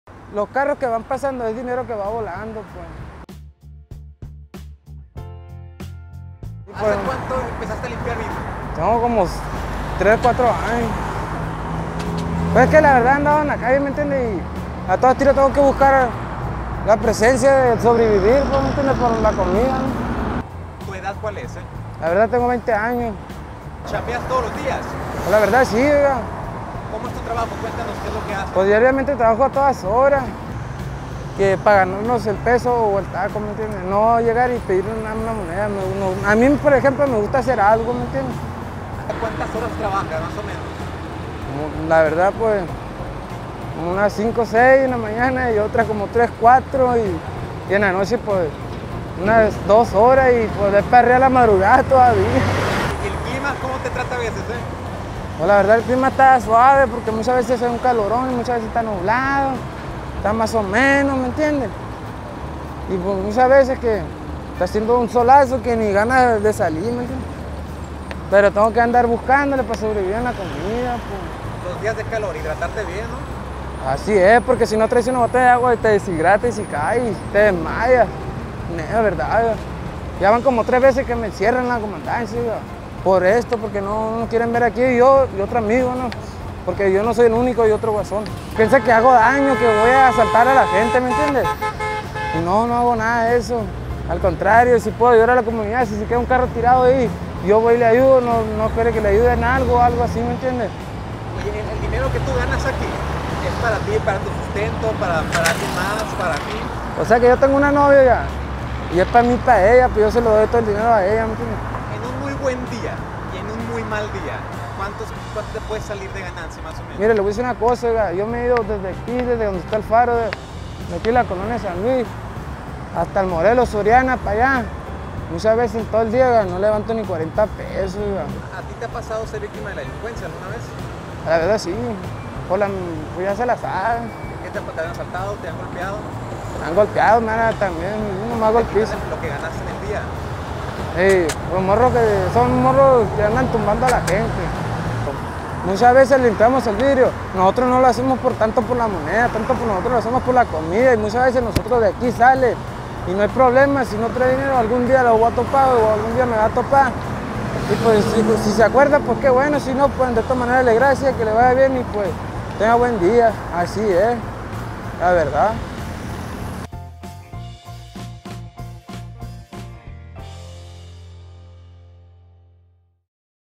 Así es el día con un limpia vidrios.
Este es el día de un limpiavidrios en Hermosillo, lo que gana y qué hace con el dinero.